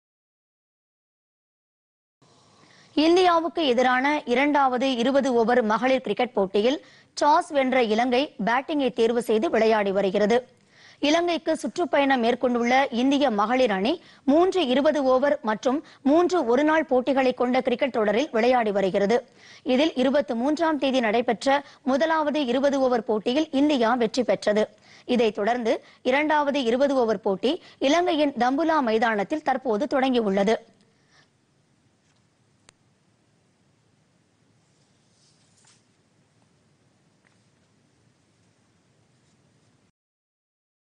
டி -20 கிரிக்கெட் - தம்புலாவில் 2 ஆவது போட்டி
இந்தியா - இலங்கை மகளிர் அணிகளுக்கு இடையிலான இரண்டாவது இருபது ஓவர் கிரிக்கெட் போட்டி-தம்புலா மைதானத்தில் தொடக்கம்